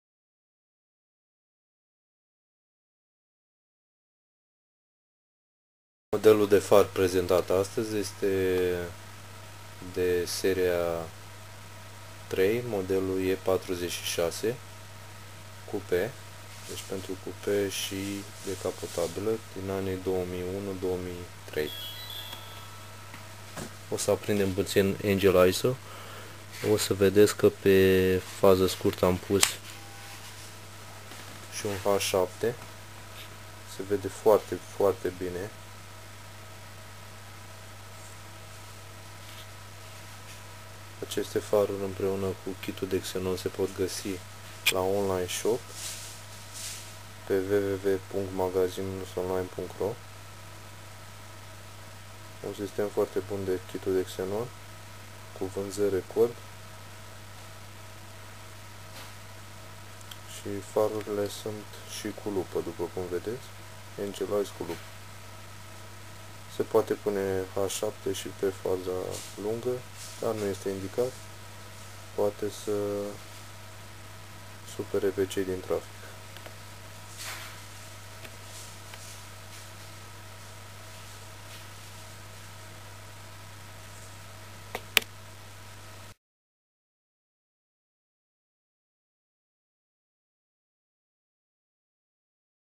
Far BMW E46 Coupe 2001 2003
Faruri cu Angel Eyes si lupa pentru BMW E46 Coupe anii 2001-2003